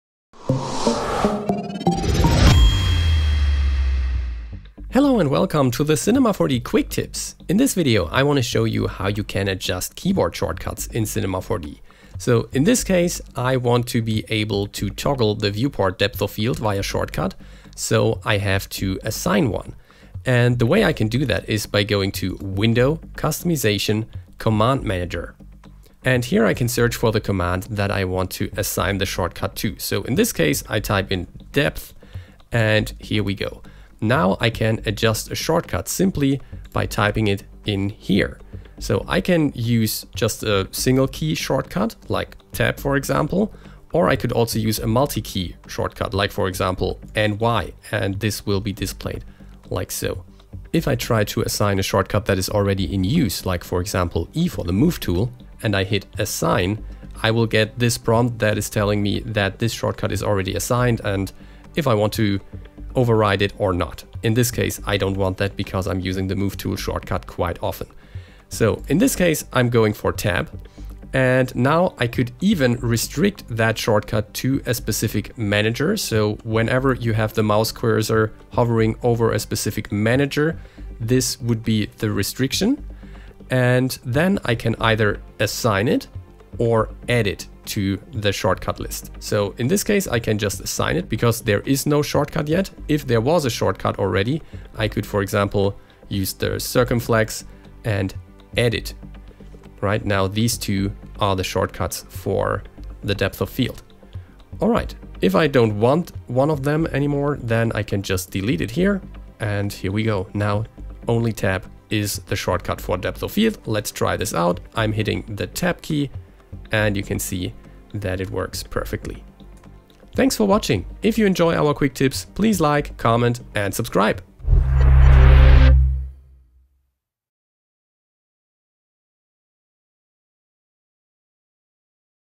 C4DQuickTip 51: How to adjust keyboard shortcuts in Cinema 4D
In this Quick Tip, Jonas Pilz (@jonaspilz3d) shows how to adjust keyboard shortcuts in Cinema 4D. You will learn how to create single-key and multi-key shortcuts as well as how to restrict them to a specific manager.

Free assets from the Cinema 4D Asset Browser used in this Quick Tip:
Levers.c4d

Recorded in Cinema 4D S26.1.